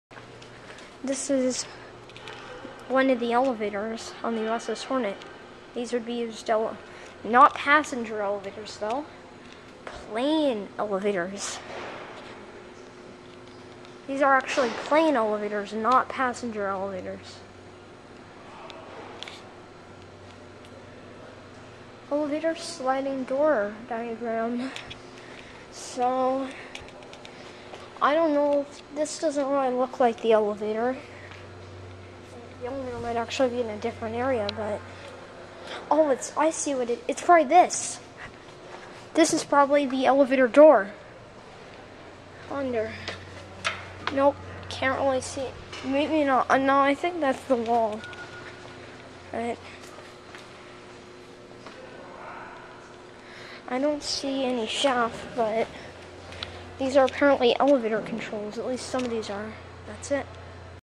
Antique/Vintage Westinghouse? Traction Aircraft Elevator 3 at the USS Hornet Museum in Alameda, CA
This elevator was moved, so this elevator is probably newer than the other elevators.
Specs:
Brand: Westinghouse?
Type: Hydraulic
Fixtures: GE
Floors: 2
Year installed: ?